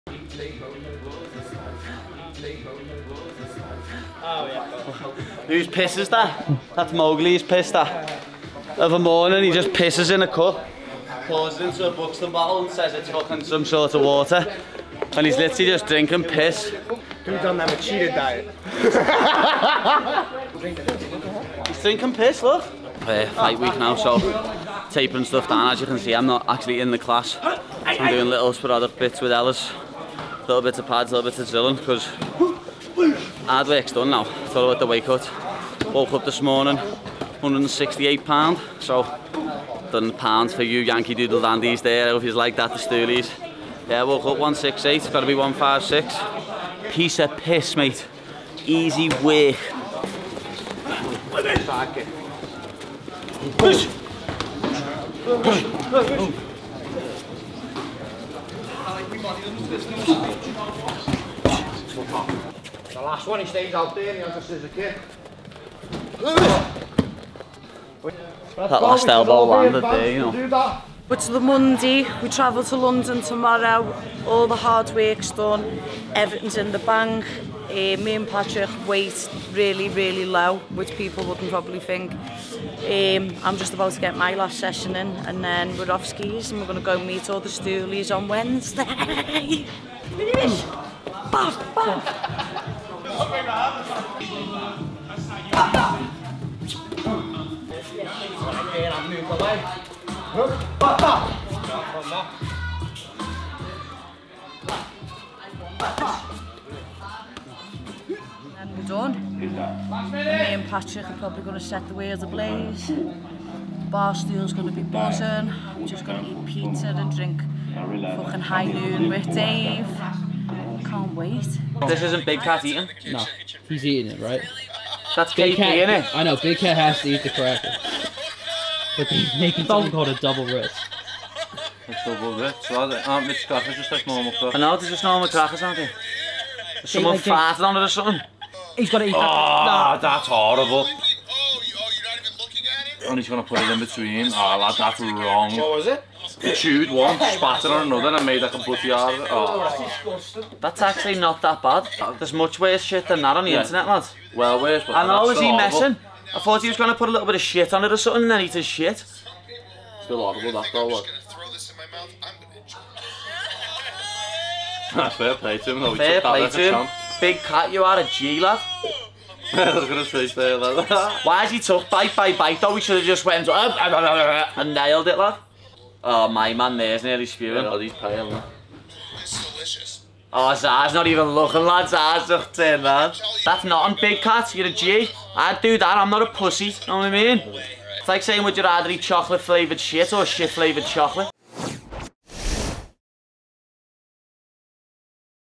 UFC London Fight Camp Diaries With Paddy The Baddy and Meatball Molly
Paddy The Baddy and Meatball Molly are checking in from UFC London with their Fight Camp Diaries.